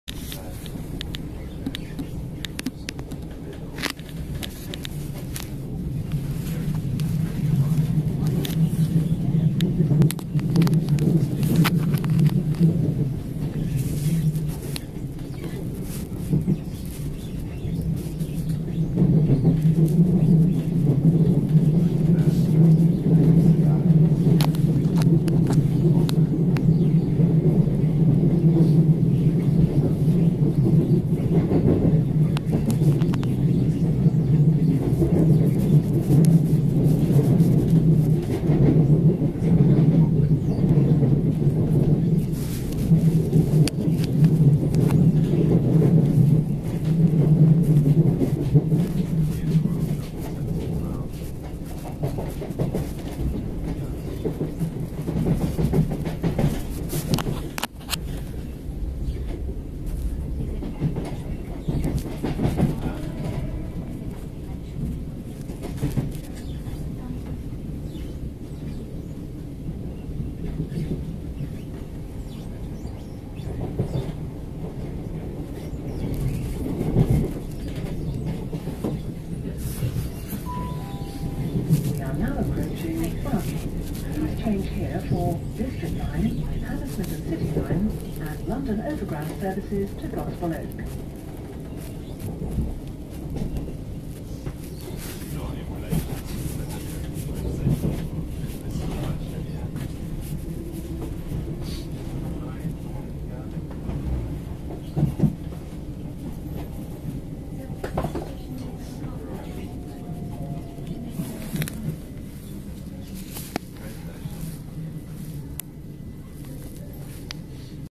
Class 357 Electrostar C2C
This is a National Rail C2C service to Shoeburyness.
Calling At - Barking, Upminster, West Horndon,Laindon, Basildon, Pitsea, Benfleet, Leigh-On-Sea,Chalkwell, Westcliff,Southend Central,Southend East, Thorpe Bay and Shoeburyness

This is Class 357 Electrostar.   Carriage Number: 74165

Tuesday 22nd December 2009